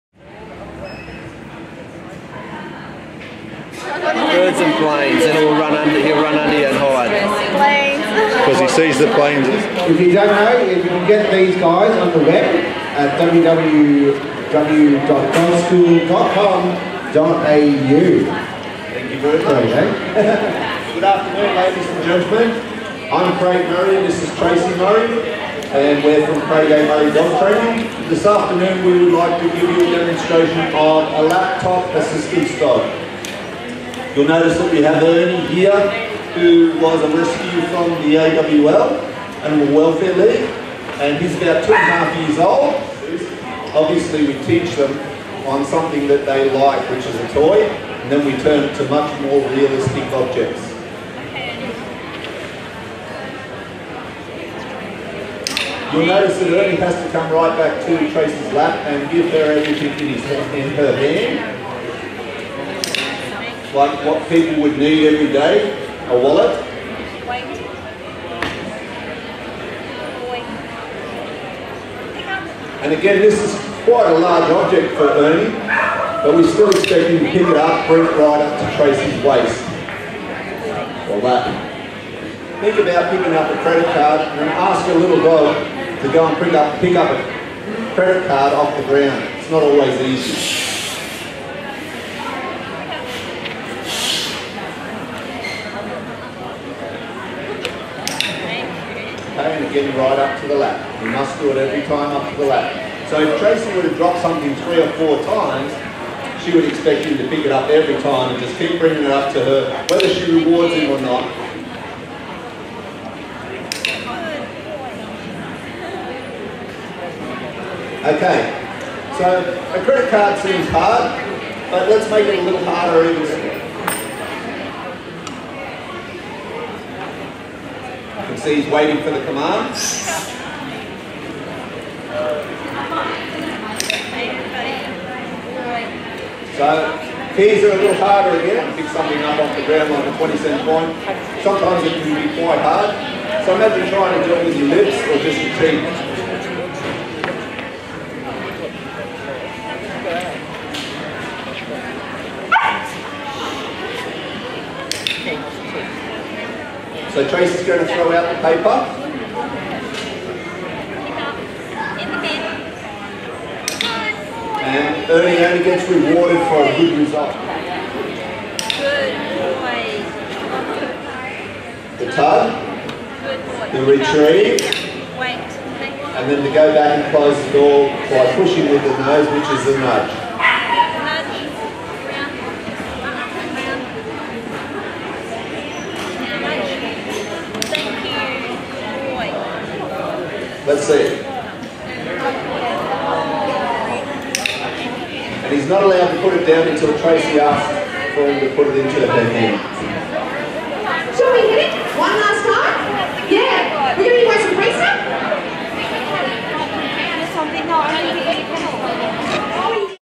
G C C C Pet & Animal Expo 2013
Craig & Tracey demonstrating a Lap Top Assistance Dog.  This dog was a rescue dog from AWLQ and rehabilitated and re-educated for assistance dog work.  He is trained to retrieve items that may be dropped by his handler and to take them back to their lap.  He can do various tasks like getting help, taking items to or picking up items from others to take back to his handler, putting things in the bin, turning on or off lights, opening and closing some doors and many more.  Just goes to show little dogs can do big things.